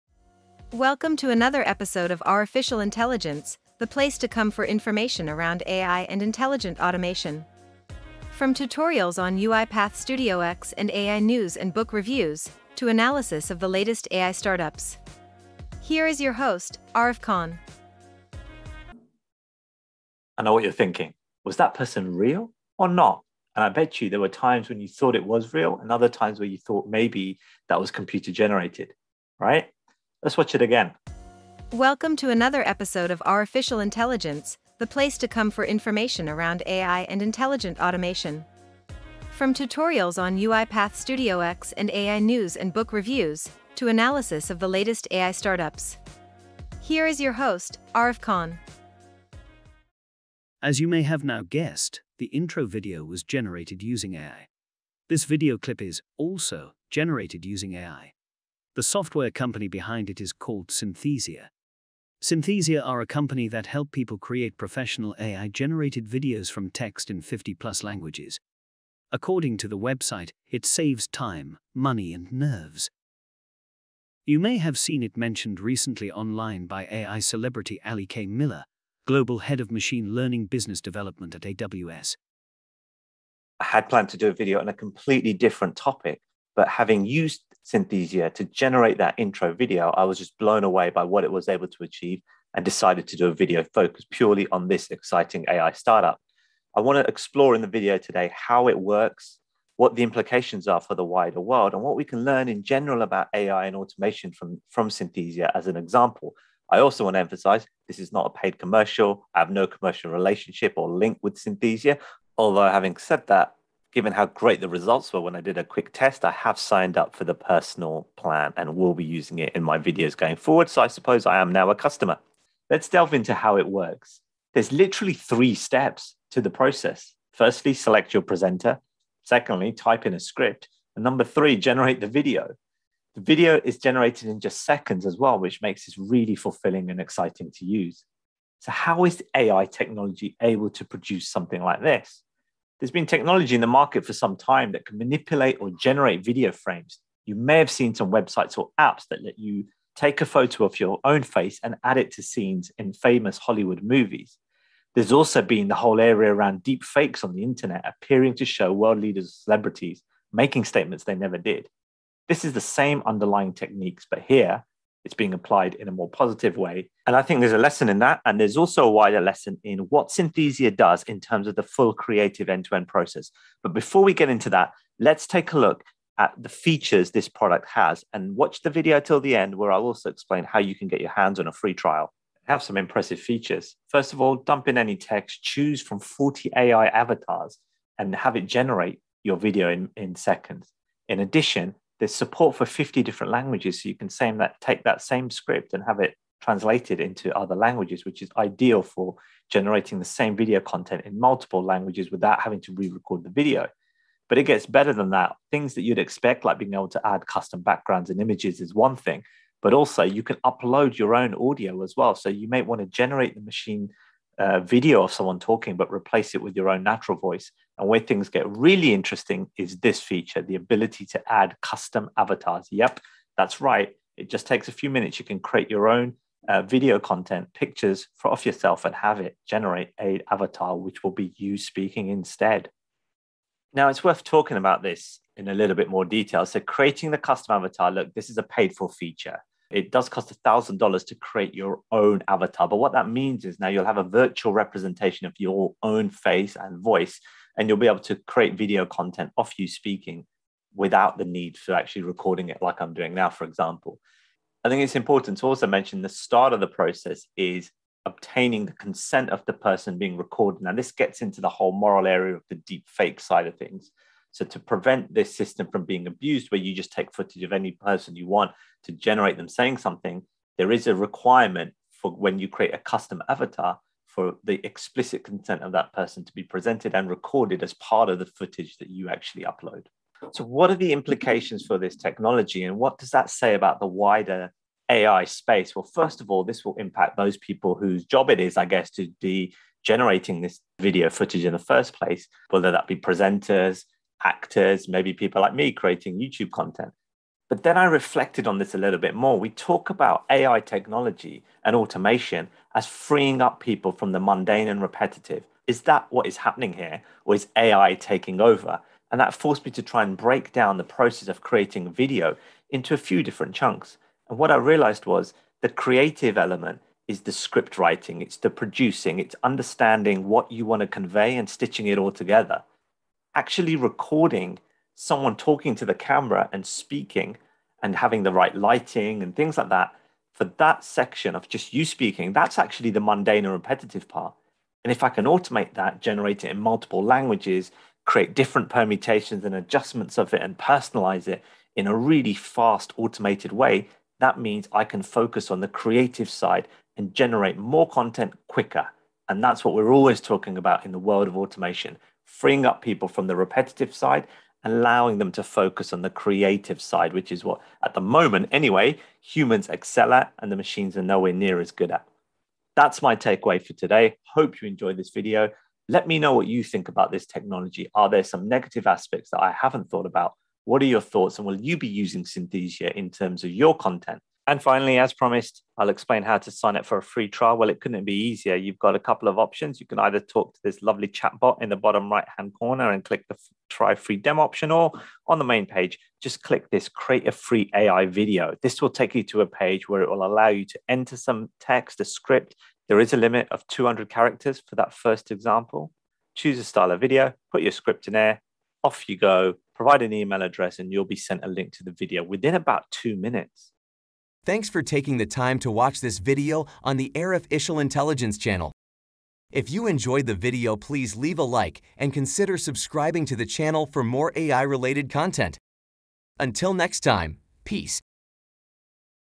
Impact of AI on Video Creation - Synthesia
Have you come across the area of Deep-fakes?

💻 What if that same technology could be used to rapidly accelerate the generation of video content? 📹

In this video I take a look at Synthesia and how they are addressing this exact need. Not only do they offer 40 avatars at your disposal to create content in 50+ languages, they also allow you to create your own custom avatars.

Is there a more general lesson here? Is AI taking over, or are Synthesia taking on the repetitive and mundane piece of content generation, so you can focus on the creative and be more productive?